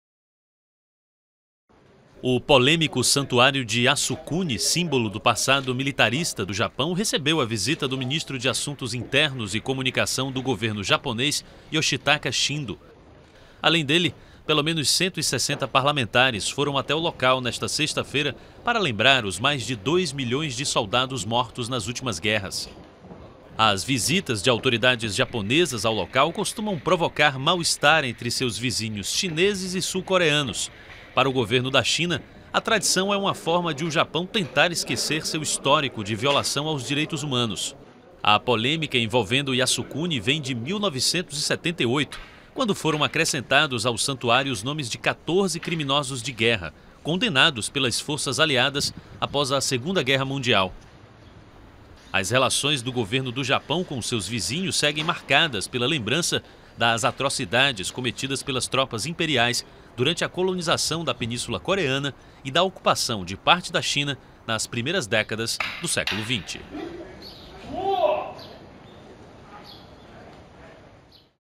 Ministro japonês visita santuário polêmico
O ministro de Assuntos Internos e Comunicação do Japão, Yoshitaka Shindo, visitou, nesta sexta-feira, o polêmico santuário xintoísta de Yasukuni, em Tóquio. O local é um marco do passado militarista japonês e as visitas costumam provocar mal-estar entre os vizinhos do país asiático.